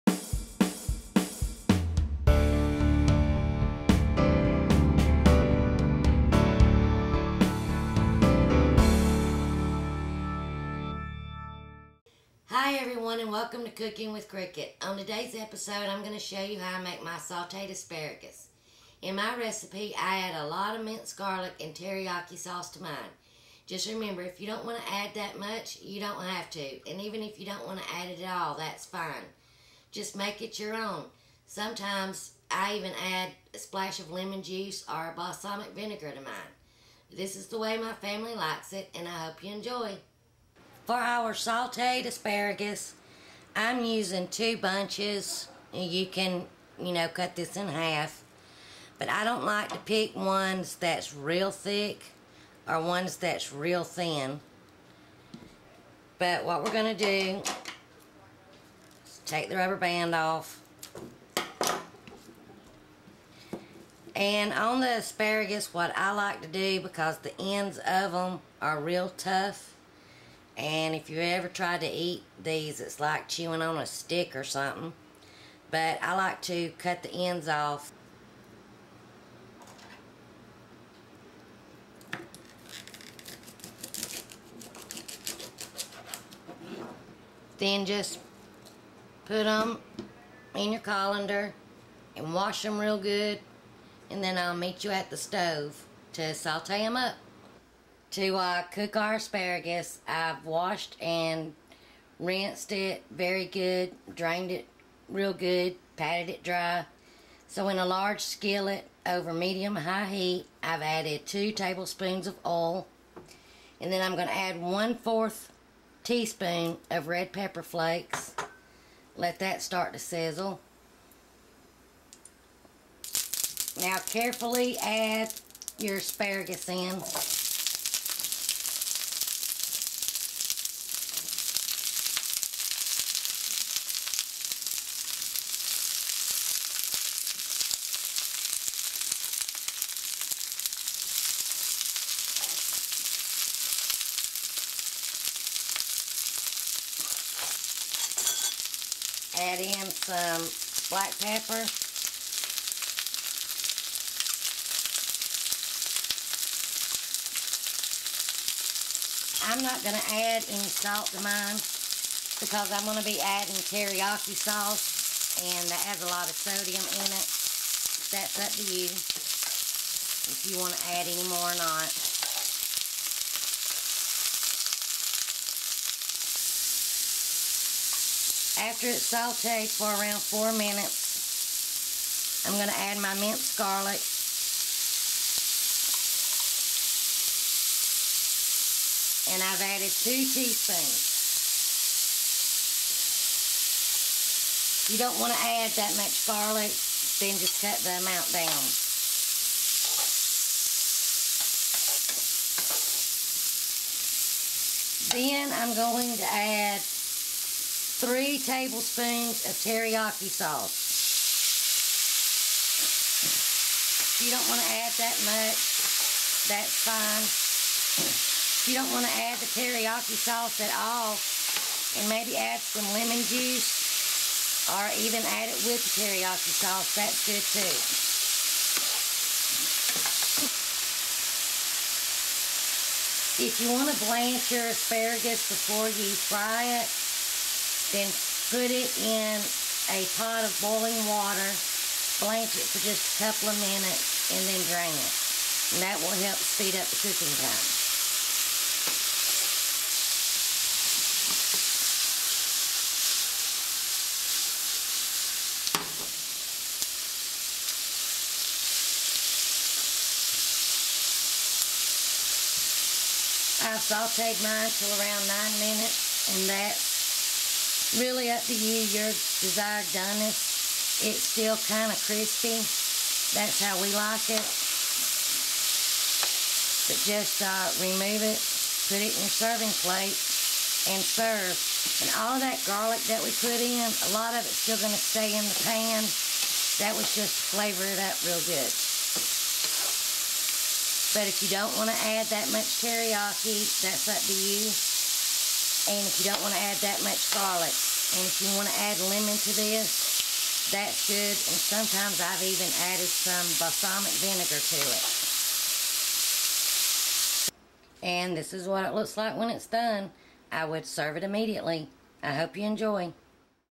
Cooking With Cricket - Sauteed Asparagus
This is how I cook my Asparagus. I'm sorry for the loud sizzling noise while cooking. Hope you enjoy.